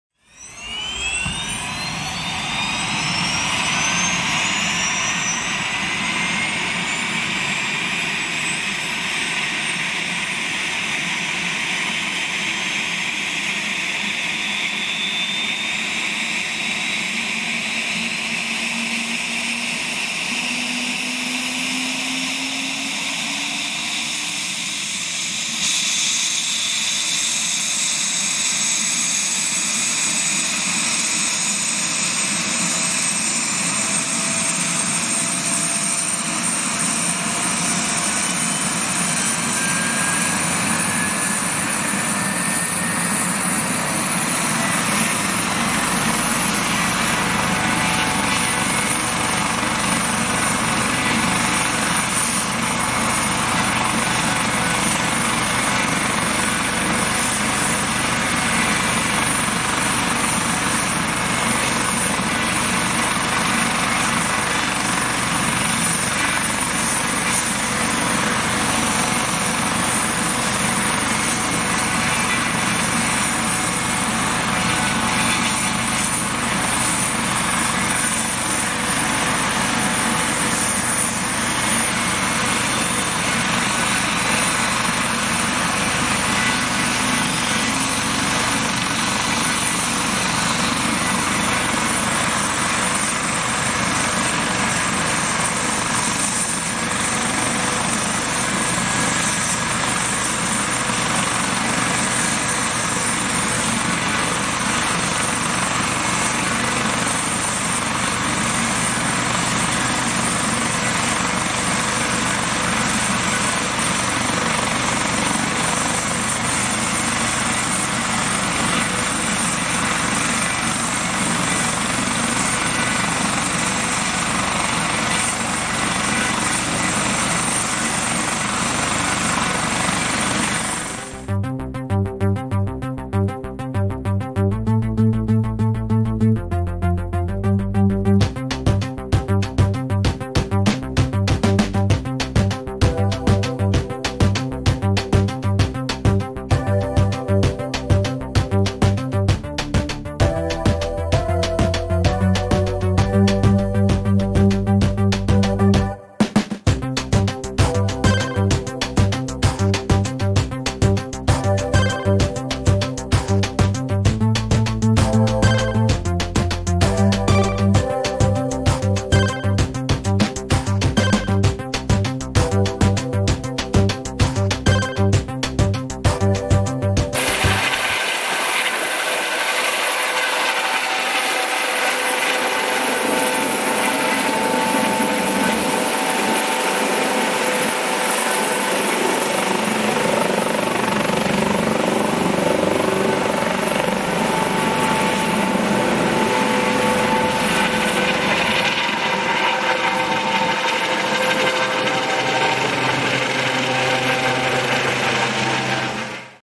Great North Air Ambulance Clitheroe
The Great North Air Ambulance on a call in Clitheroe, Lancashire.